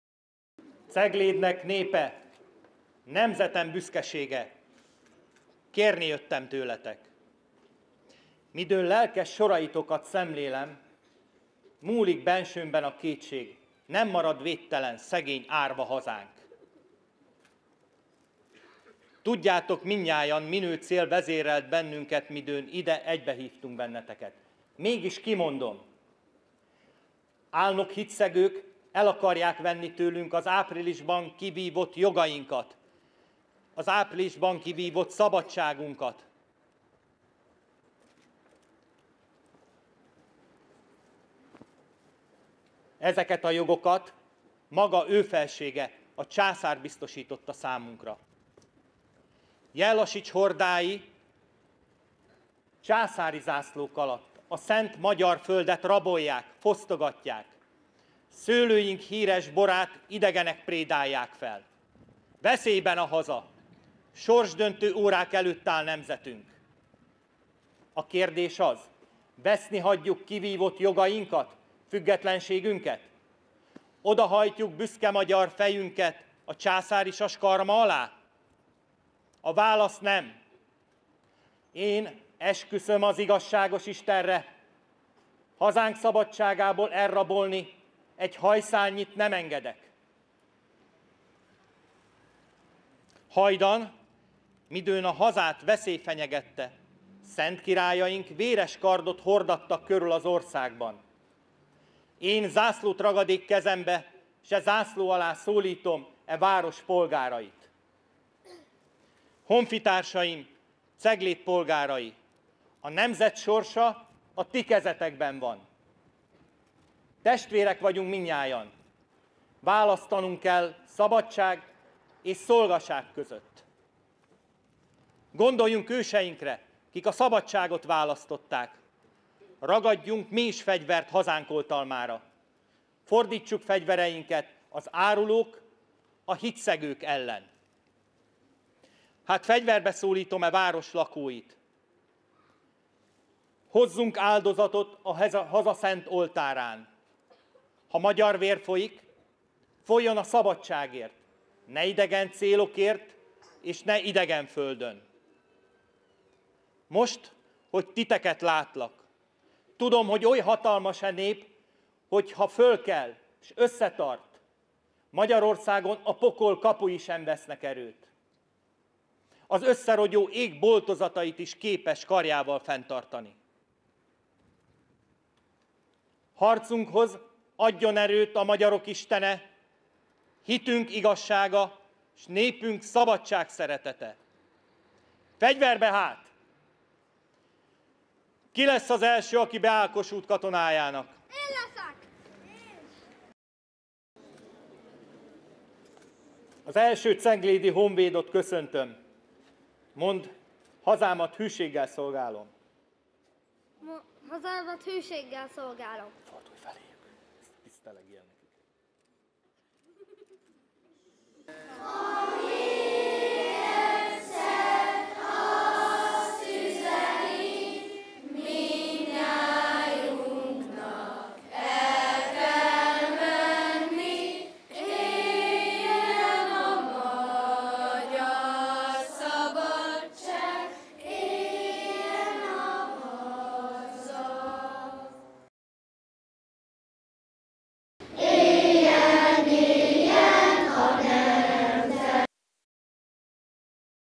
Kezdetét vette Kossuth Lajos ceglédi toborzóbeszédének évfordulójára rendezett ünnepi sorozat
Adás: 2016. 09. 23. Háttér magazin - CTV

Több száz gyerek együtt énekelt Kossuth nótákat ma délelőtt. A téren ezzel vette kezdetét a néhai államférfi ceglédi toborzóbeszédének évfordulójára rendezett ünnepi sorozat. Az éneklő ifjúságot a Táncsics Mihály Általános Iskola énektagozata vezette.